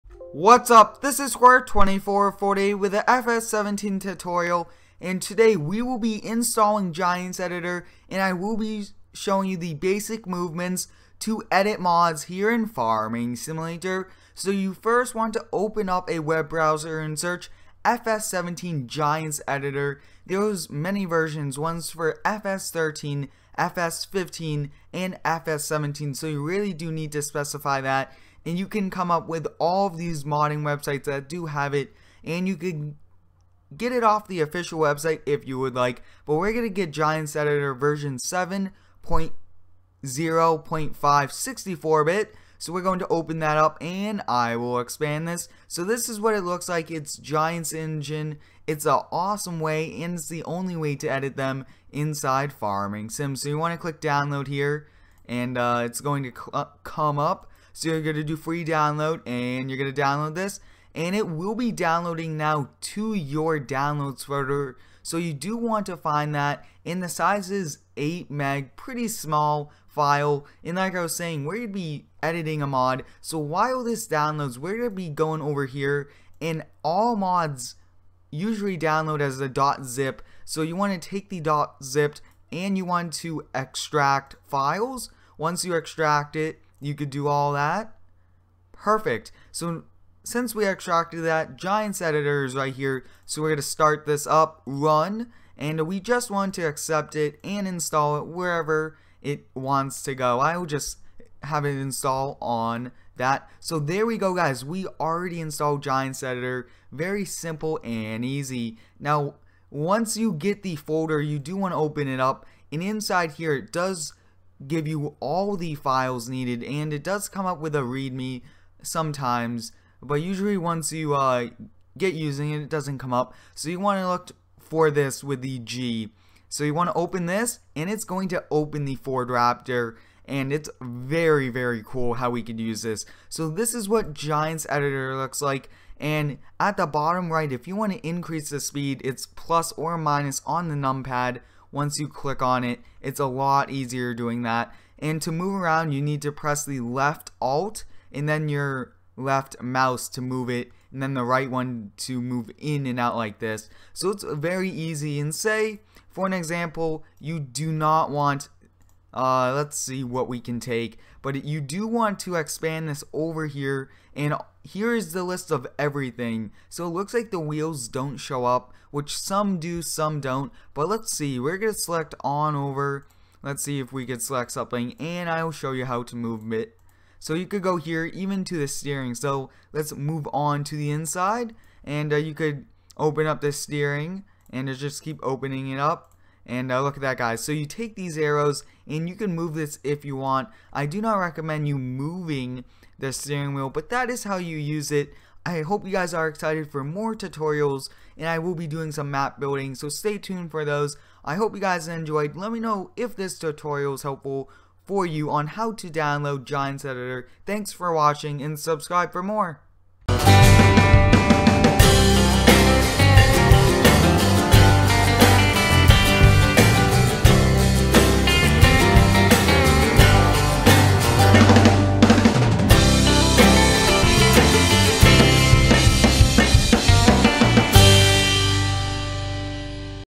FS17: Tutorial - How To Install Giants Editor
Today we install Giants Editor. With this program we can edit Mods!

Thanks, Square2448